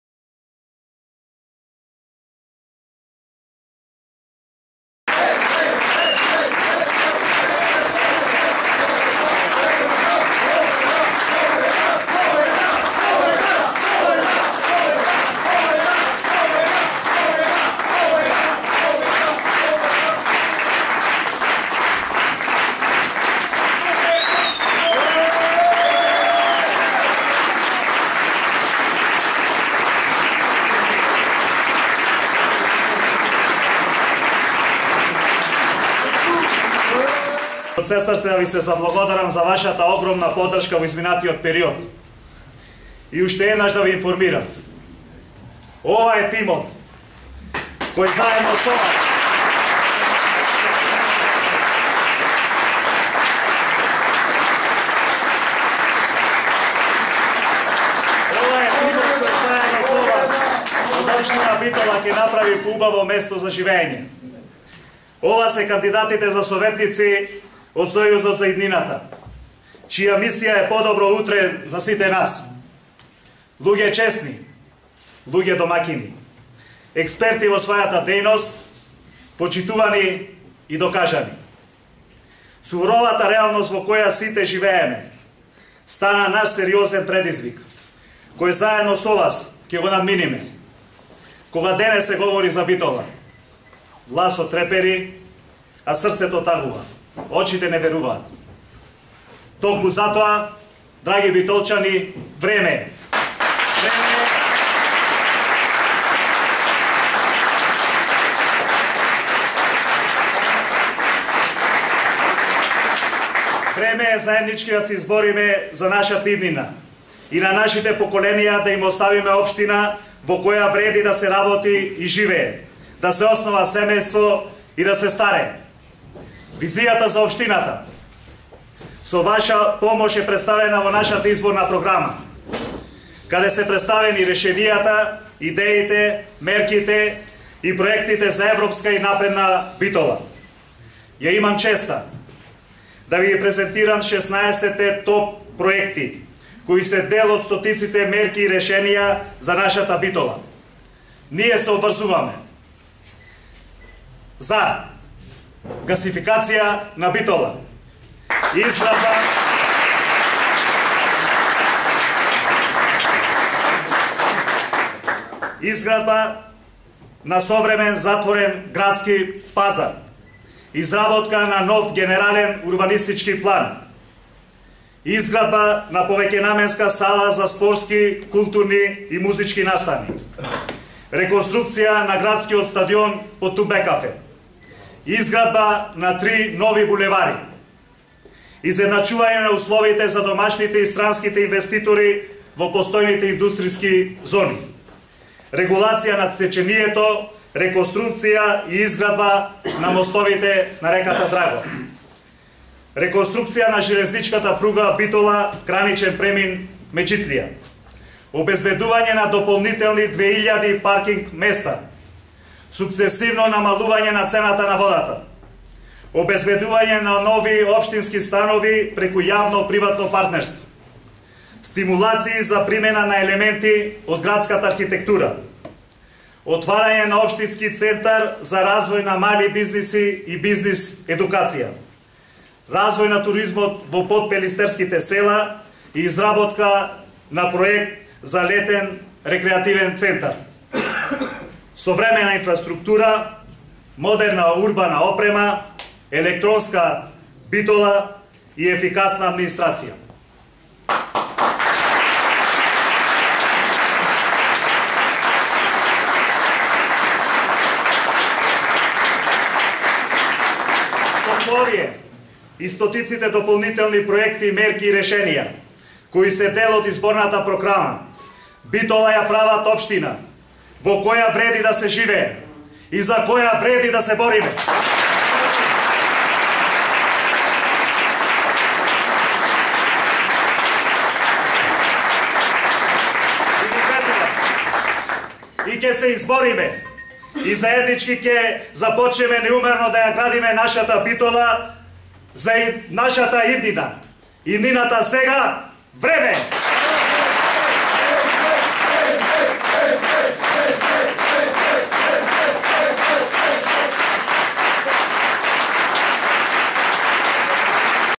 Mende Dinevski so 16 proriteti za poubava Bitola
Идниот Градоначалник на Општина Битола - Менде Диневски со 16 приоритети за поубава Битола